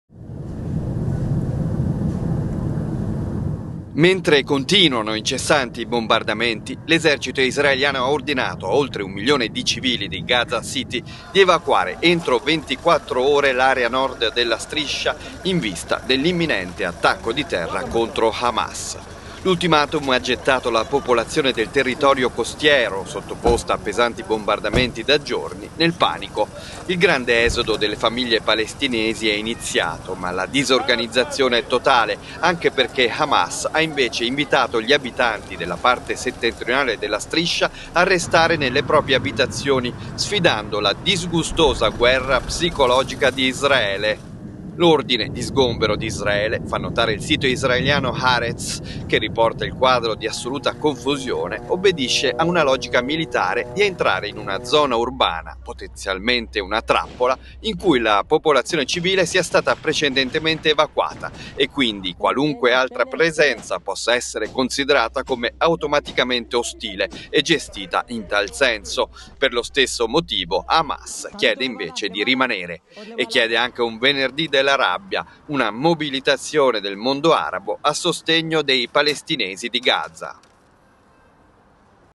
L'ordine di sgombero di Israele getta Gaza nel caos
Milano, 13 ott. (askanews) - Mentre conutinuano incessanti i bombardamenti, l'esercito israeliano ha ordinato a oltre un milione di civili di Gaza City di evacuare entro 24 ore l'area nord della Striscia in vista dell'imminente attacco di terra contro Hamas.
L'ultimatum ha gettato la popolazione del Territorio costiero, sottoposta a pesanti bombardamenti da giorni, nel panico.
Il grande esodo delle famiglie palestinesi è iniziato ma la disorganizzazione è totale anche perché Hamas ha invece invitato gli abitanti della parte settentrionale della Striscia a restare nelle proprie abitazioni sfidando la "disgustosa guerra psicologica di Israele".
L'ordine di sgombero di Israele, fa notare il sito israeliano Haaretz che riporta il quadro di assoluta confusione, obbedisce a una logica militare di entrare in una zona urbana - potenzialmente una trappola - in cui la popolazione civile sia stata precedentemente evacuata e quindi qualunque altra presenza possa essere considerata come automaticamente ostile e gestita in tal senso; per lo stesso motivo, Hamas chiede invece di rimanere.
E chiede anche un venerdì della rabbia, una mobilitazione del mondo arabo a sostegno dei palestinesi.